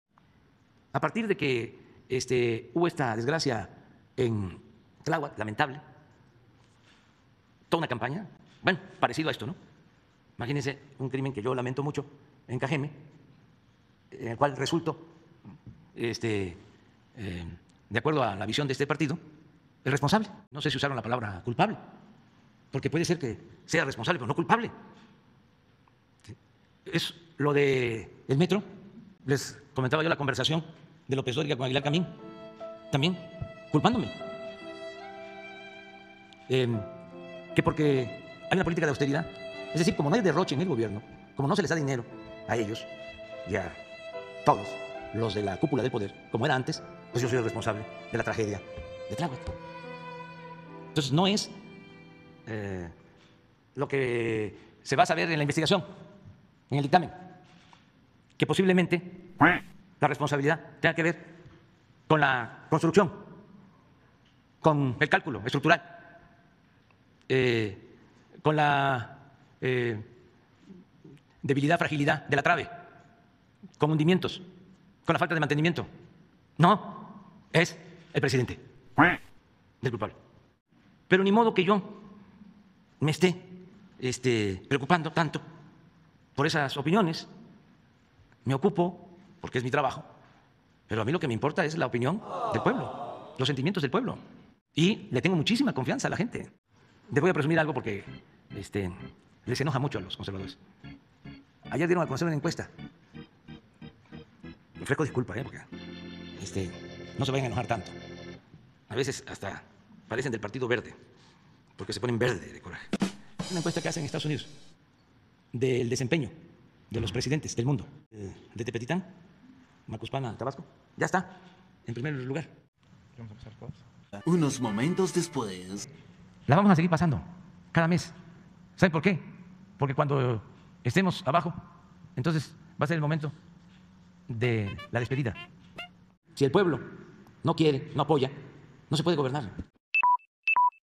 Hoy habló de la Linea12delMetro pero para victimizarse porque muchos lo señalan como responsable 🙄
Hoy habló de la Linea12delMetro pero para victimizarse porque muchos lo señalan como responsable 😢... y cerró presumiendo una encuesta que lo coloca en 1er lugar de los mandatarios del mundo 🦆